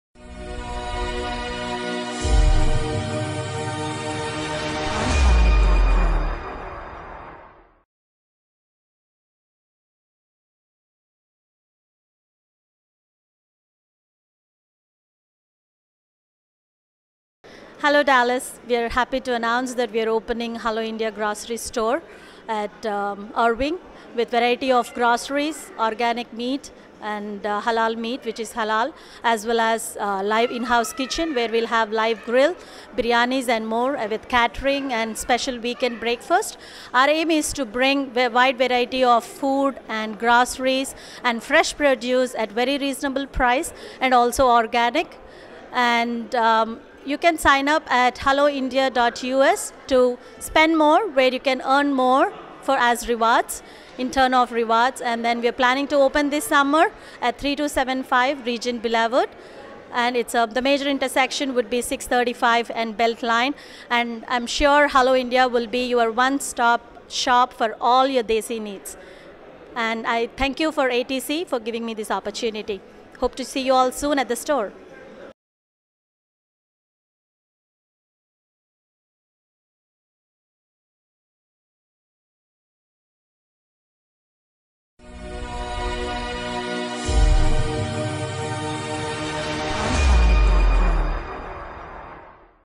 Hello India - New Grocery Store in Irving - America Telugu Convention - Dallas - Texas
Visuals from ATC - American Telugu Convention in Dallas texas, held from May 31st to June 2nd at Irving Convention Center in Dallas, Texas. USA